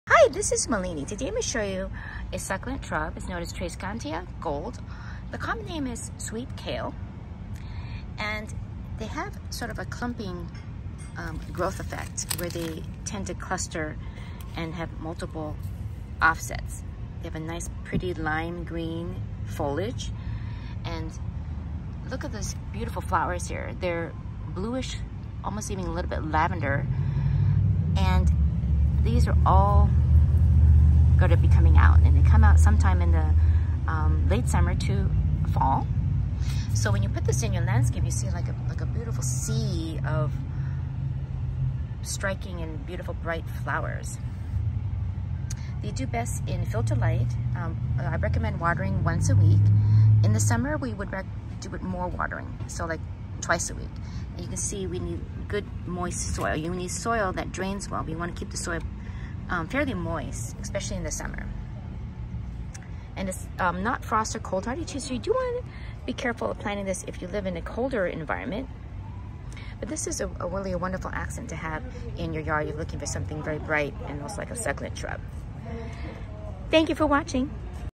Succulent shrubs add beauty and bright color: Tradescantia "Gold" or "Sweet Kale" 😍👍💐💙💚❤️
Origin:
Flower:  Blue-Lavender flowers that can continue flowering if old blooms removed.
Description: A fast growing succulent shrub with lime green branching leaves.  Plant can reach up to 12 feet tall. Has a clumping growth form. Flowers attract butterflies.
Sun:  Filtered sun
Water:  Once a week
Frost:  Not cold or frost hardy.